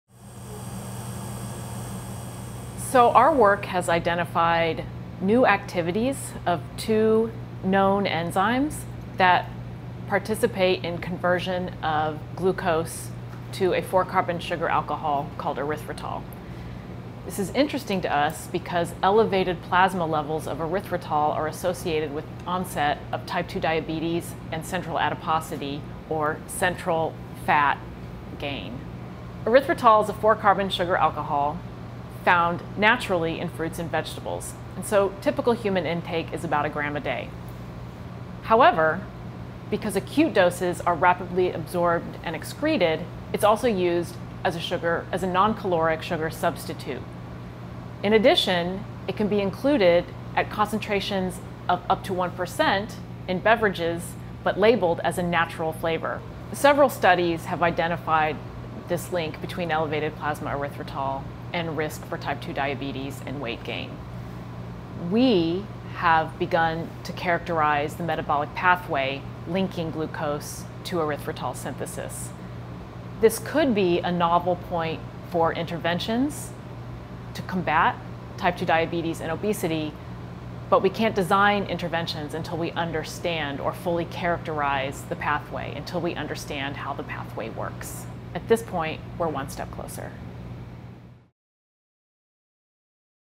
Understanding the metabolic pathways to obesity and diabetes
Martha Field, an assistant professor in the College of Human Ecology, is studying the metabolic pathways that lead to the creation of erythritol, a byproduct of the body’s synthesis of glucose. Understanding this metabolic path could open an array of possibilities for beating obesity, heart disease and type 2 diabetes.